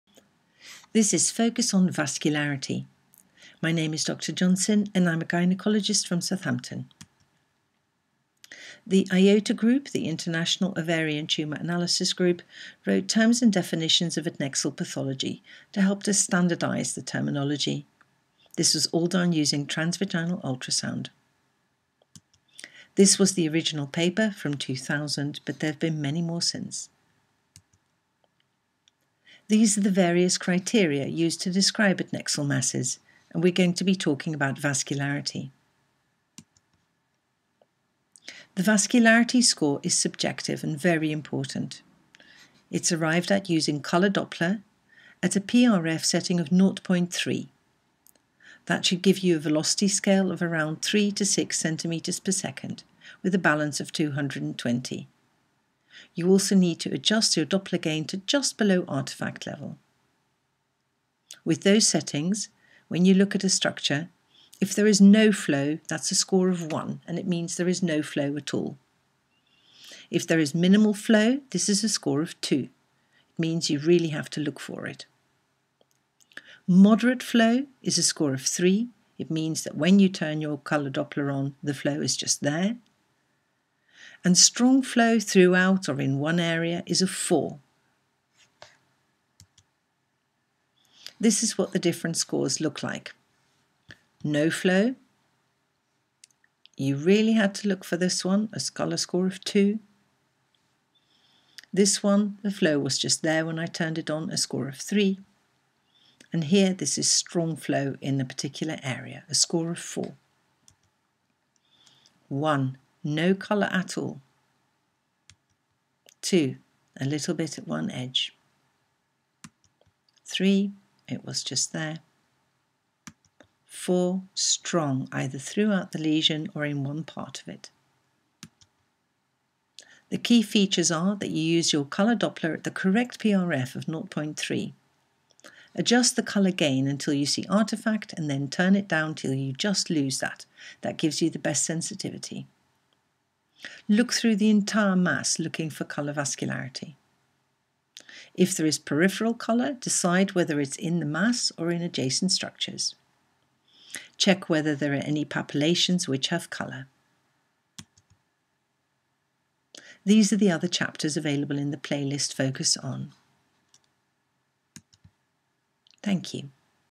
4 Vascularity
This video contains detailed information from the IOTA classification system for adnexal masses.